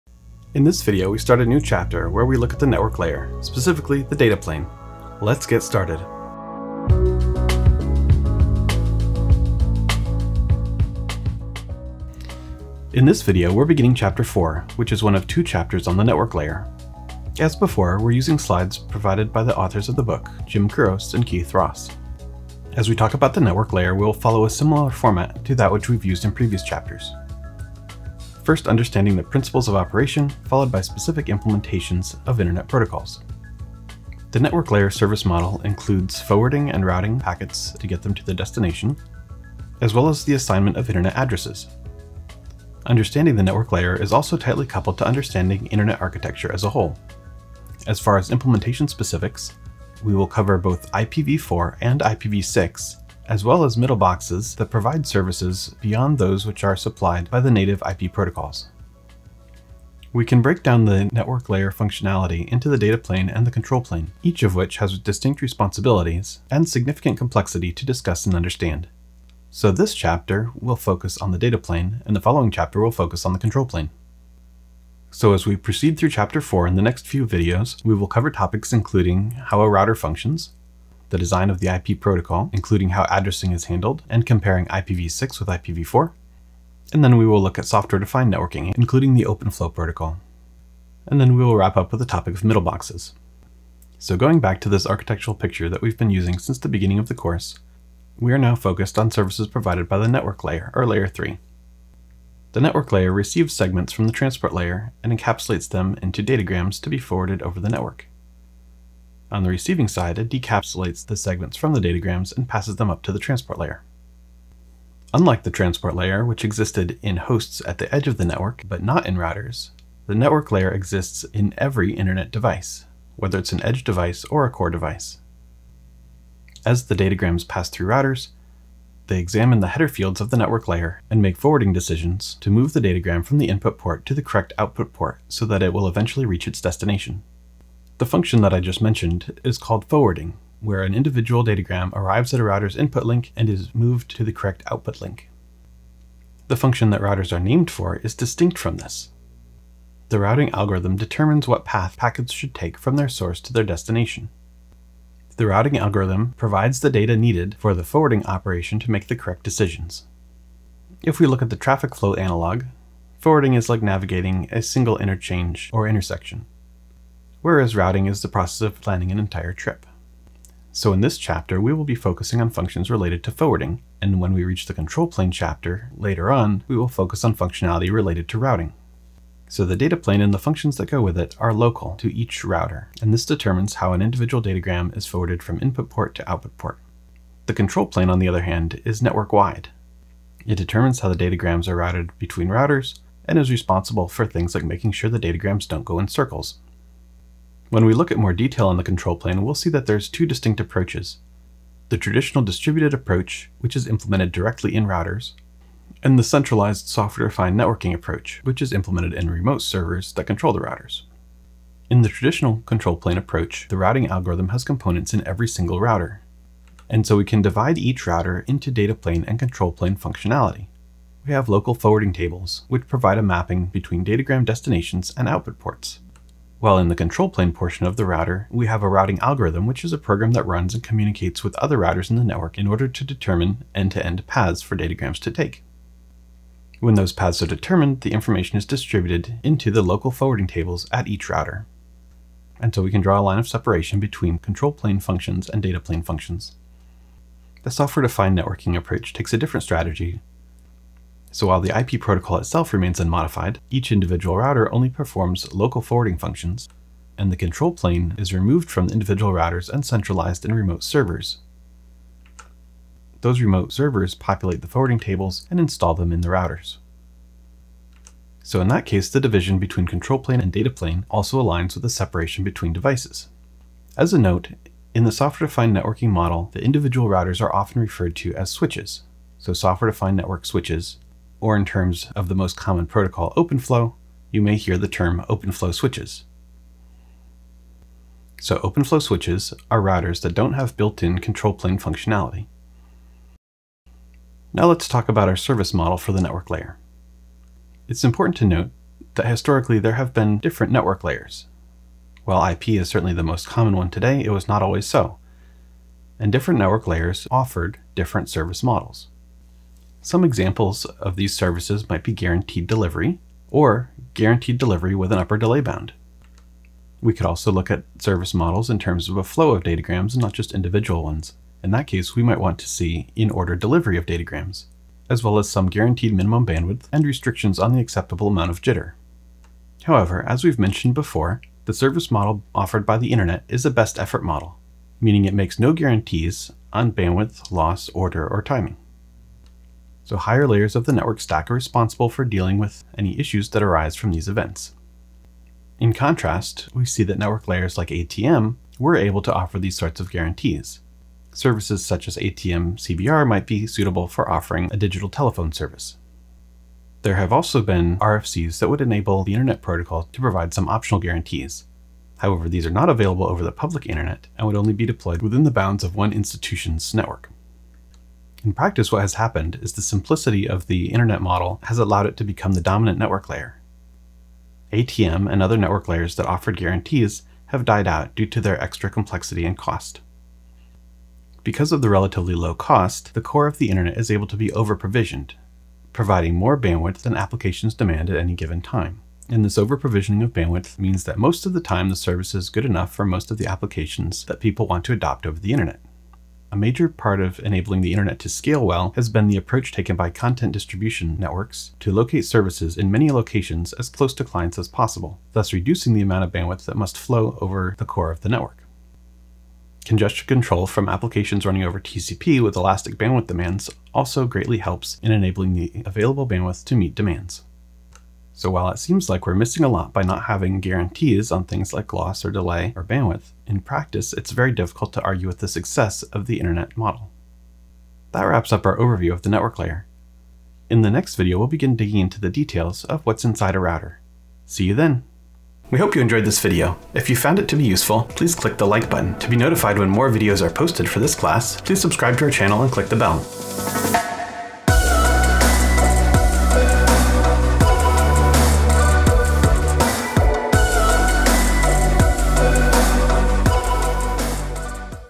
Overview of the Internet Protocol - IP Network Layer | Computer Networks Ep. 4.1 | Kurose & Ross
Answering the question:  "What does the network layer do?"  Discusses routing vs forwarding.  Introducing the network-layer data plane.
Based on Computer Networking: A Top-Down Approach 8th edition, Chapter 4, Section 1.